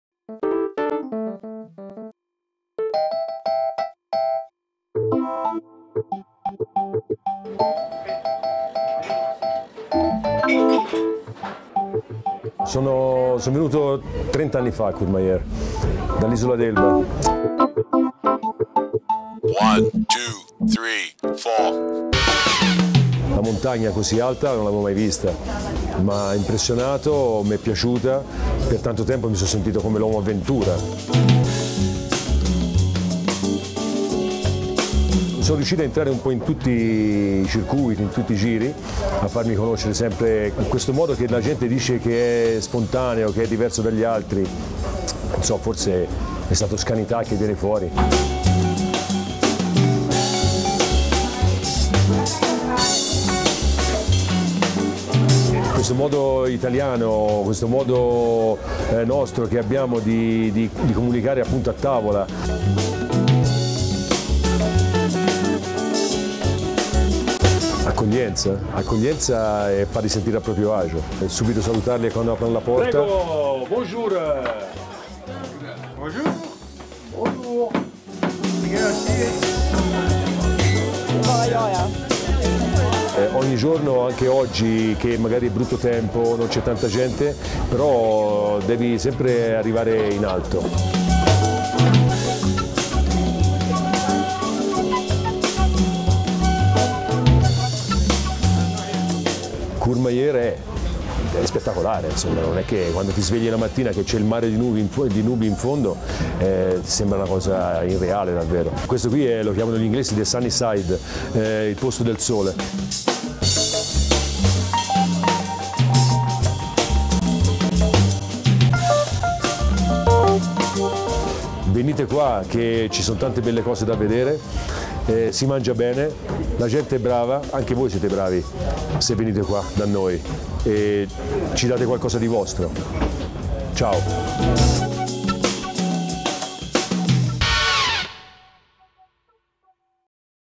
Giacomo Calosi, CourmaPeople i Greeters di Courmayeur
Giacomo Calosi, per tutti Giacomo della Maison Vieille, è il gestore del frequentatissimo Rifugio
sulle piste di Courmayeur Mont Blanc. Arrivato 30 anni fa dall'isola d'Elba ha scelto Courmayeur per trasmetterci la sua passione nella Cucina e nell'Accoglienza.